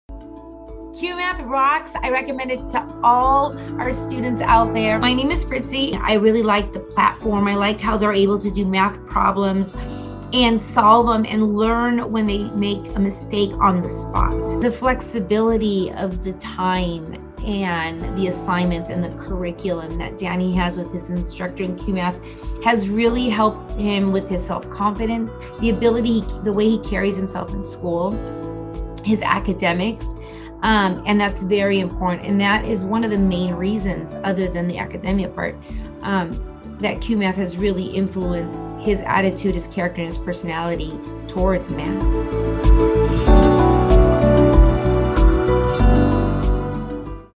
"Cuemath has changed my child's attitude towards math" | Parent Review
Listen to Fritzie's Cuemath experience as she shares how our program helped her son Danny gain confidence and excel at math.

Ready to transform your child's math skills?